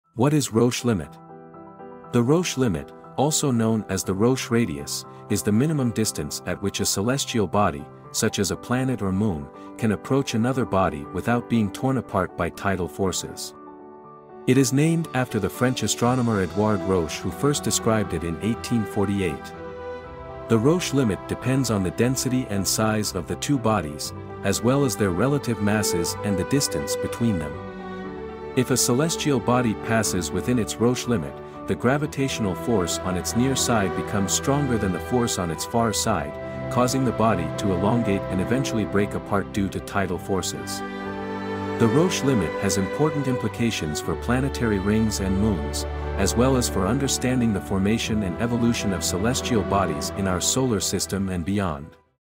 What is Roche limit?
The Roche limit, also known as the Roche radius, is the minimum distance at which a celestial body, such as a planet or moon, can approach another body without being torn apart by tidal forces. It is named after the French astronomer Édouard Roche who first described it in 1848.

The Roche limit depends on the density and size of the two bodies, as well as their relative masses and the distance between them. If a celestial body passes within its Roche limit, the gravitational force on its near side becomes stronger than the force on its far side, causing the body to elongate and eventually break apart due to tidal forces. The Roche limit has important implications for planetary rings and moons, as well as for understanding the formation and evolution of celestial bodies in our solar system and beyond.